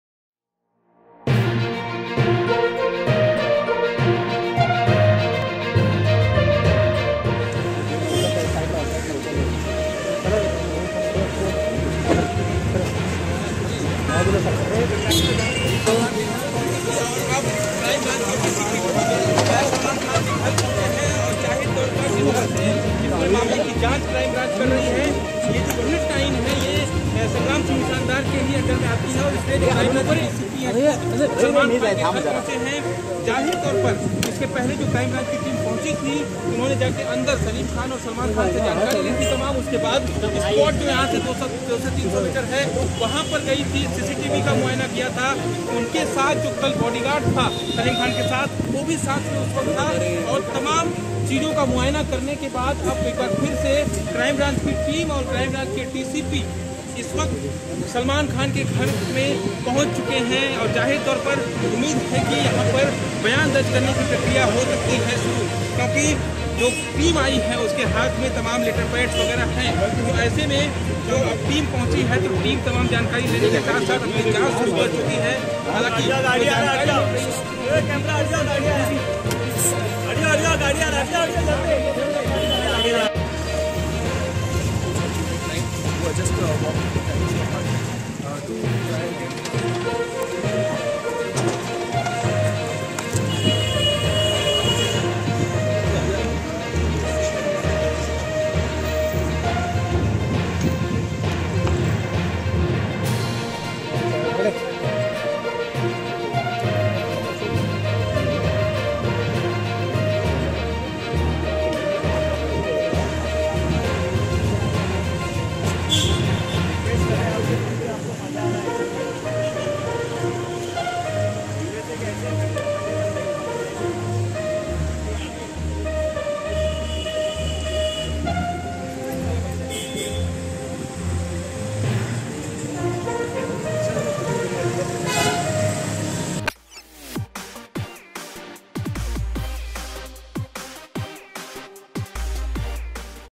Security Inspection is Going At Salman Khan’s House, Galaxy Aparment | Boogle Bollywood
Copyrights@BoogleBollywood